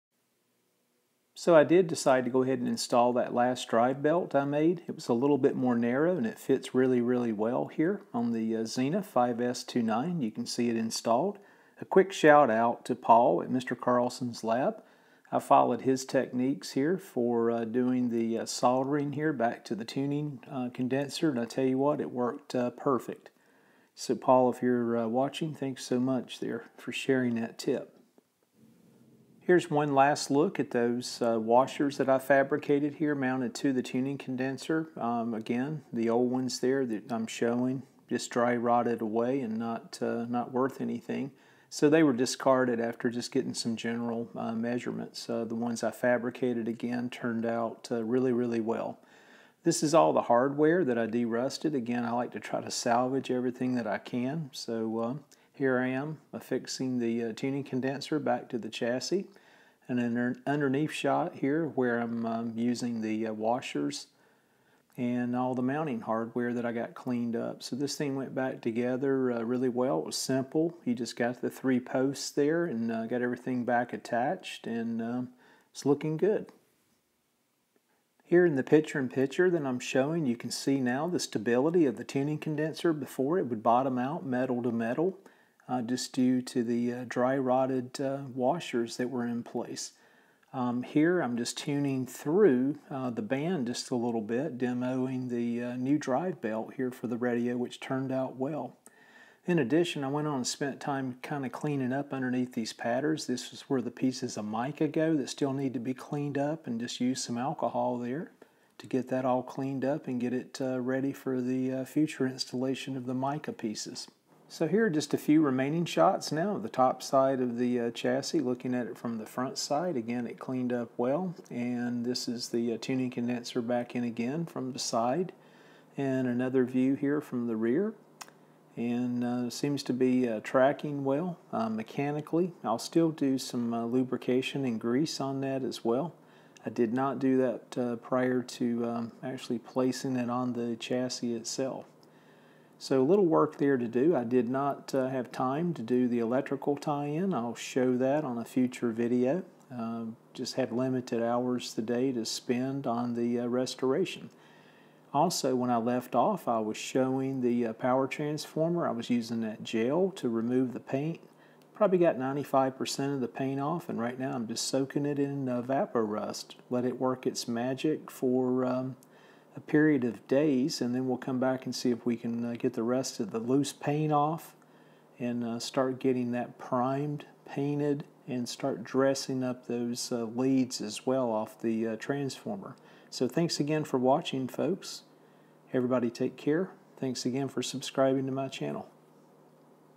Zenith 5-S-29: Tuning Condenser Remounted
I didn't have much time on my hands, but did have an opportunity to remount the tuning condenser to the chassis and solder the new leads to the tuning condenser. Thanks to Paul (Mr Carlson's Lab) for sharing his technique on how to solder directly to a chassis or other... His technique worked perfect!